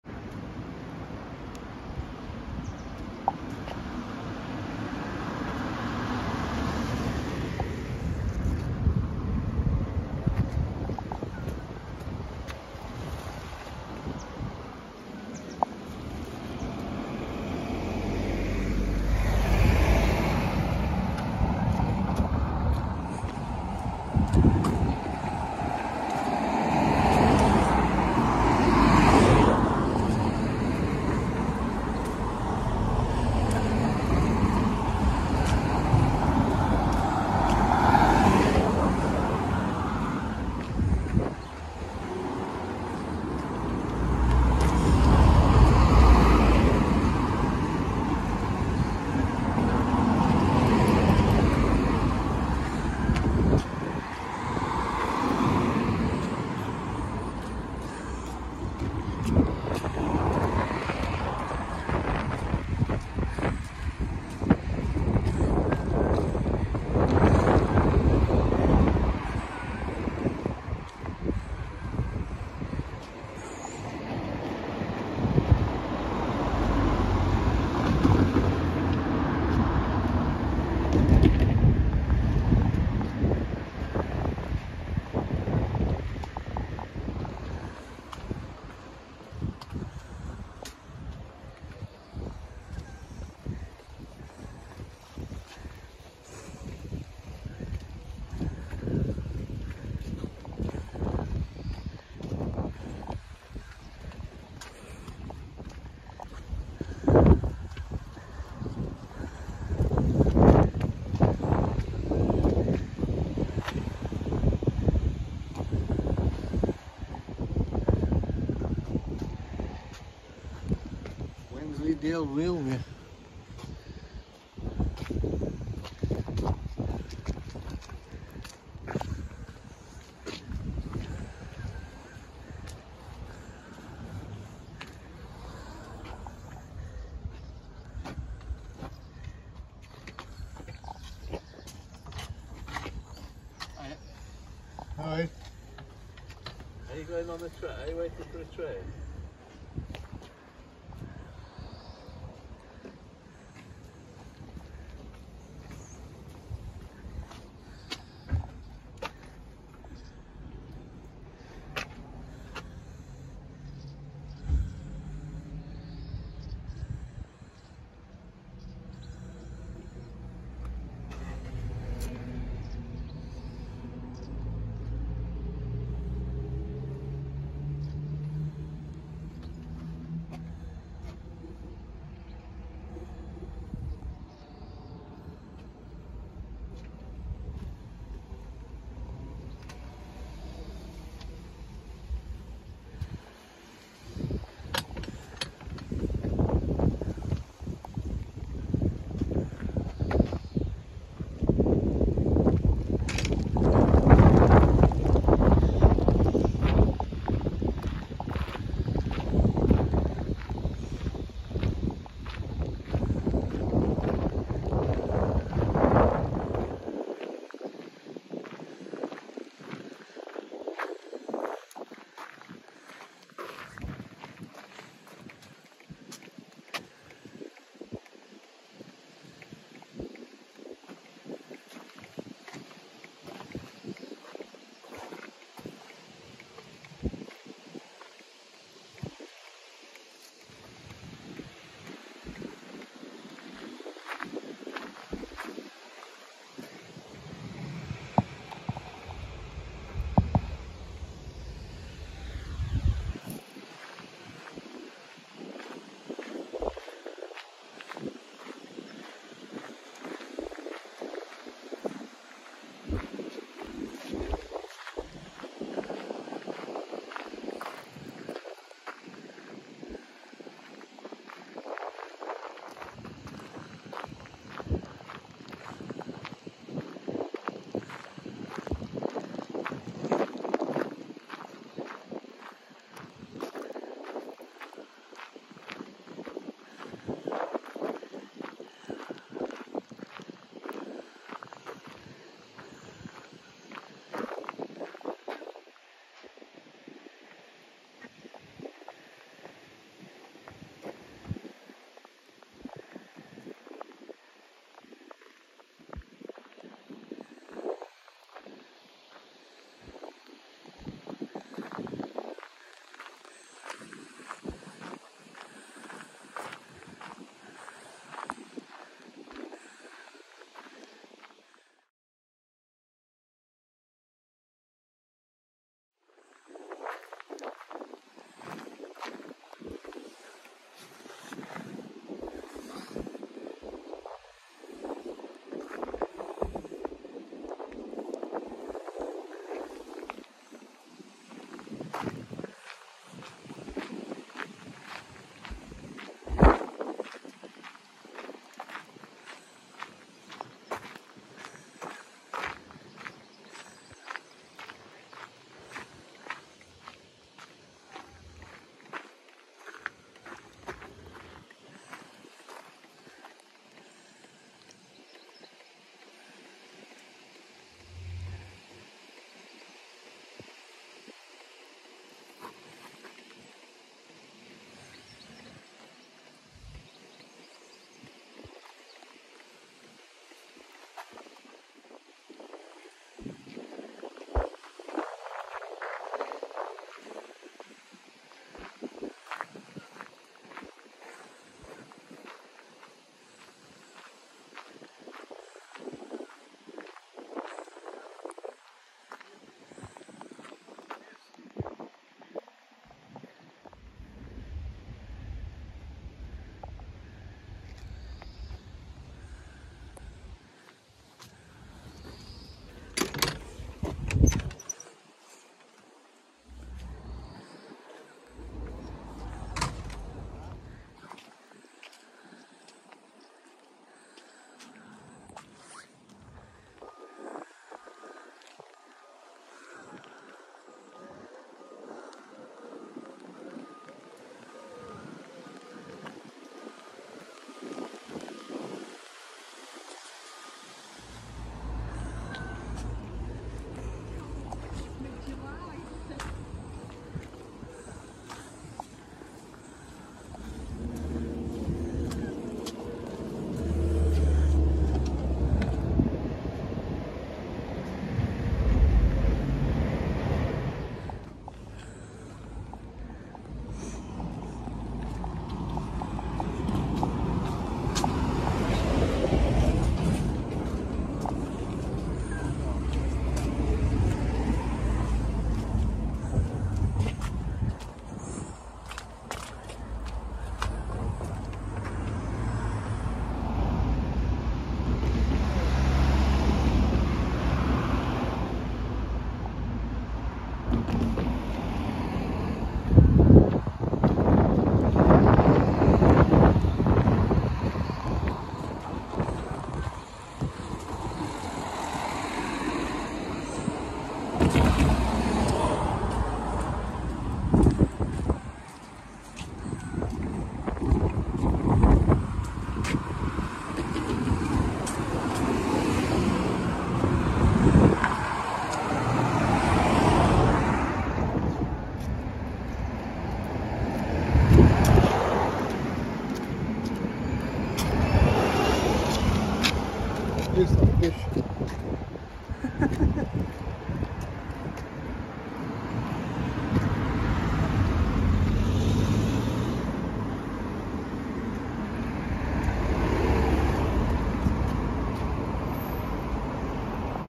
A look around Bedale train station
5 mins walk from the main high street of ​bedale is this lovely train station